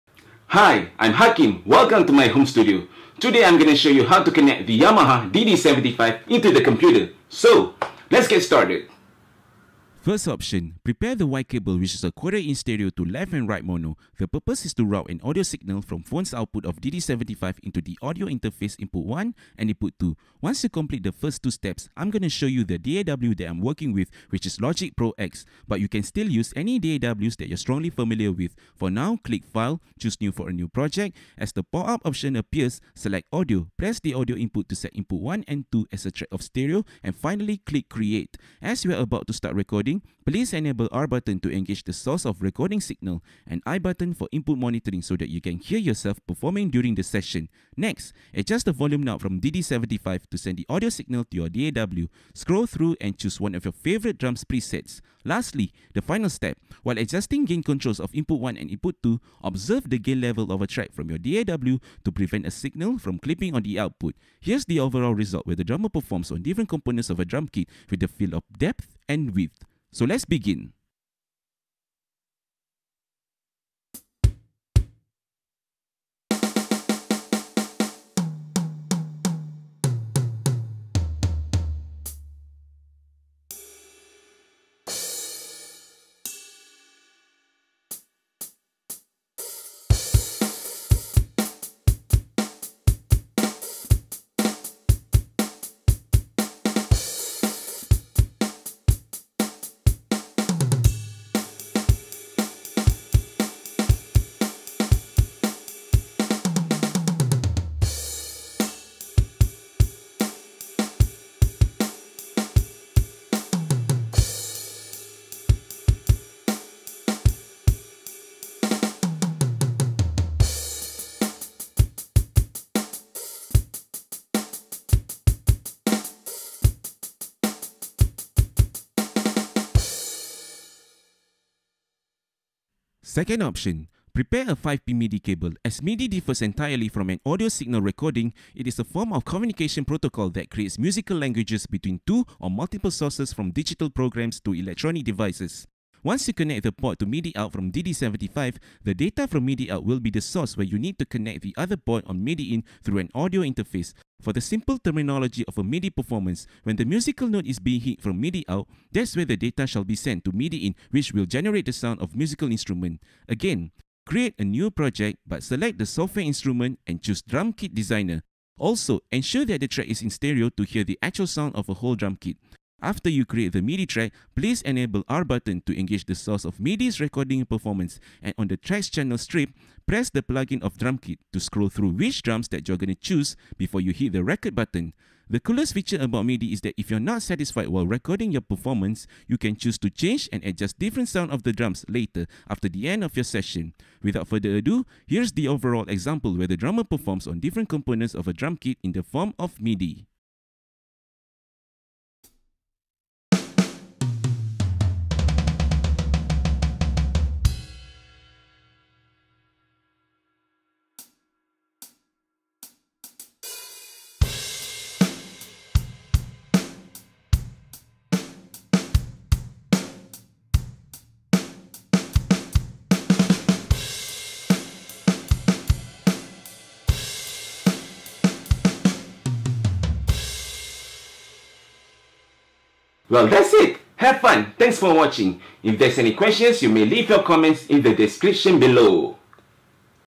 Yamaha DD75 - Drums' Recording Tutorial (Audio & Midi) Using Cable Splitter & MIDI Cable
Here we are, folks! As I promised, the tutorial video for drums' recording is finally out! Enjoy!

The content is to showcase on how the drummers can record their performance without an actual acoustic drum kit for music production.

Current gears (audio/video) that are being used for tutorial:

1. Focusrite Scarlett 2i4 (2nd Gen)
2. Yamaha DD-75
3. Logic Pro X
4. IPhone Xs Max
5. Panasonic GH5
6. Klotz 1/4” Cable Splitter
7. MIDI Cable
8. Final Cut Pro X
9. Rode VideoMic Pro
10. Shure SM58